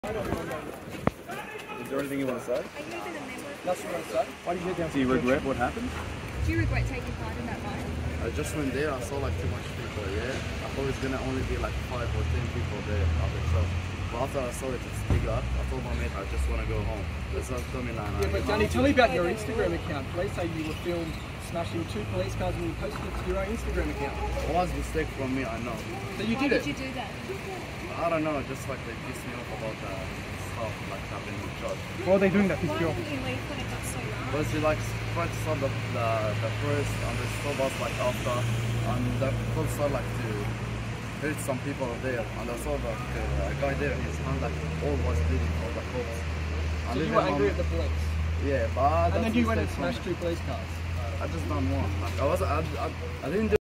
Accused church rioter says he only kicked one cop car
A teenager accused of taking part in a violent riot and damaging two police cars after the stabbing of a Sydney bishop says he made a mistake but he was angry about officers' violence. Video via AAP.